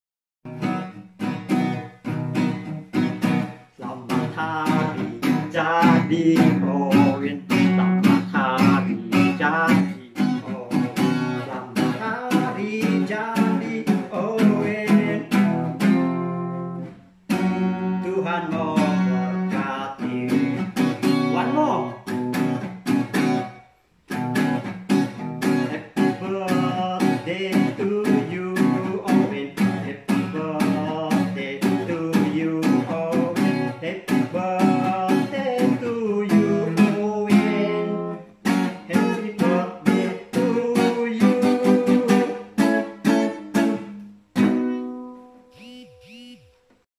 UWENSIWARA GUITARIST birthday party at hometown
Only 1set KFC with price MYR 10.40. I was bought at RANAU KFC TOWN.. thankgiving